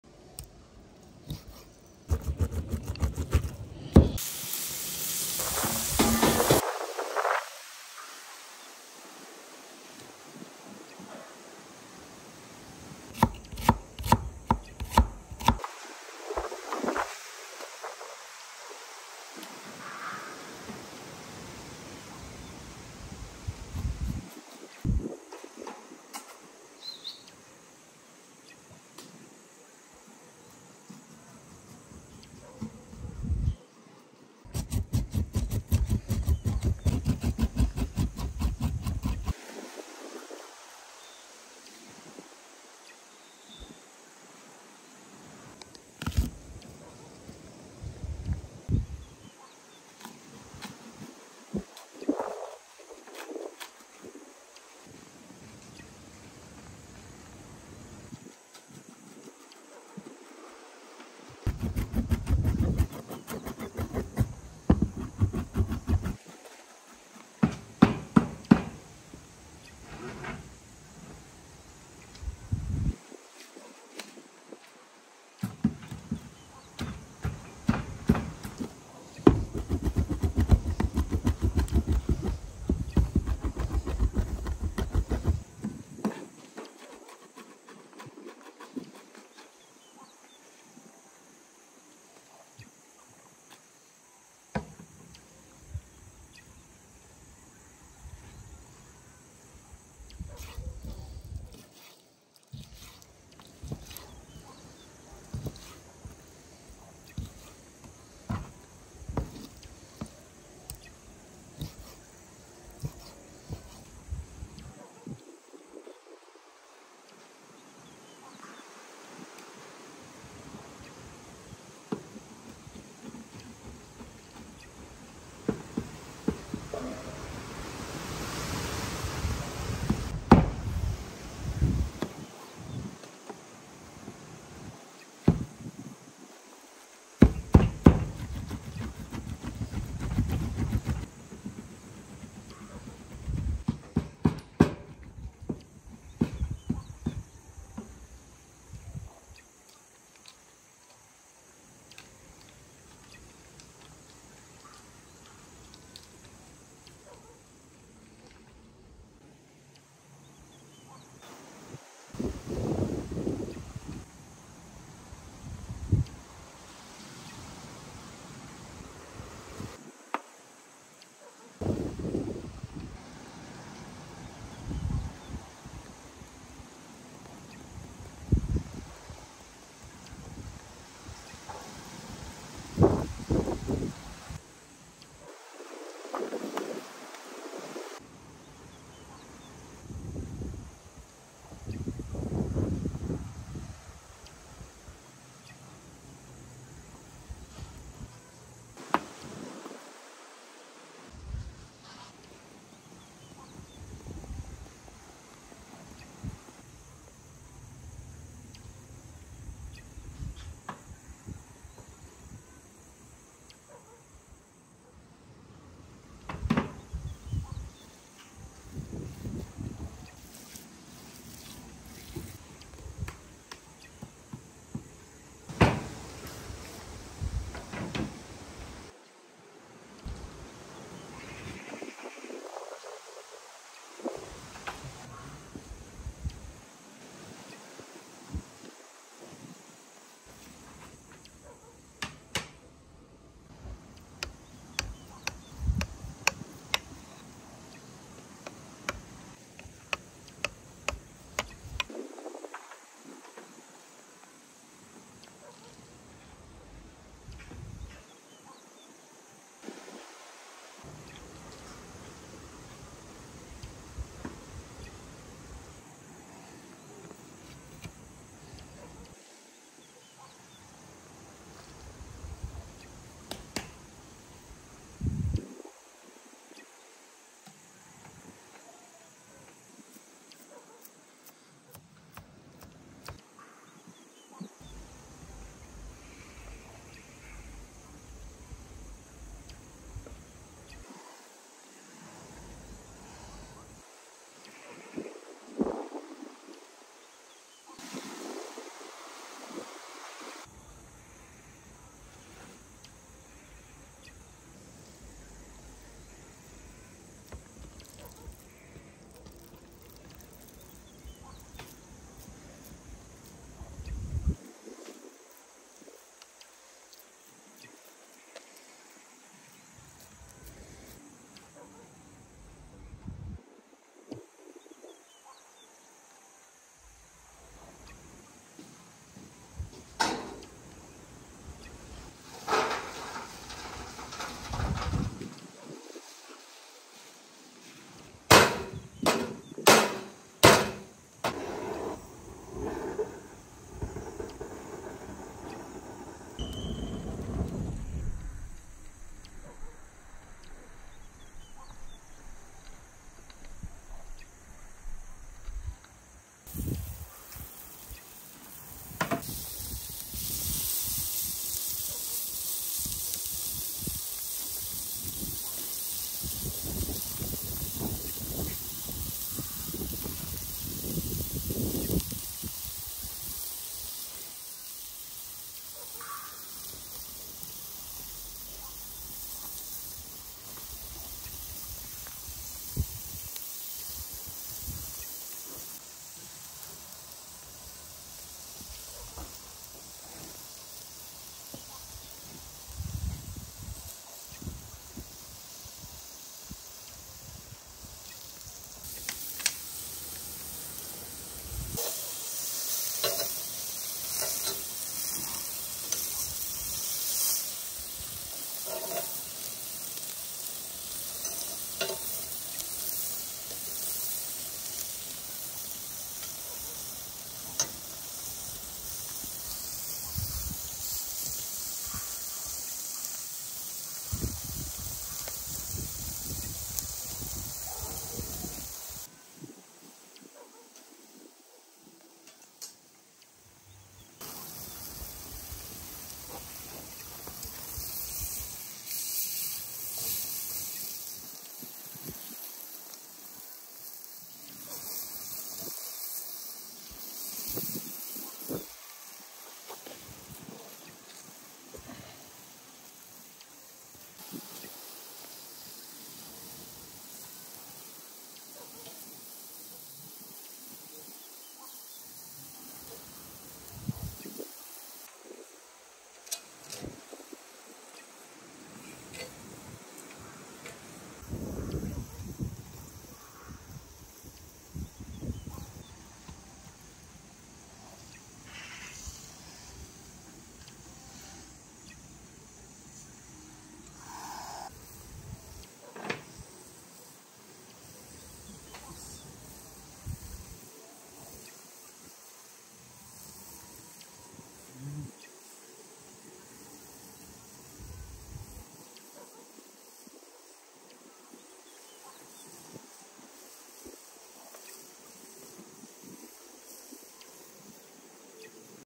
GRILLED MINCED CHICKEN with CHEESE🧀food
GRILLED MINCED CHICKEN with CHEESE🧀

👉 This dish can be made with minced chicken and you can put any kind of food that you like inside.

Ingredients: boneless chicken, cheese, chicken ham, onion, bell pepper, butter
Recipe: Black pepper powder, garlic powder, salt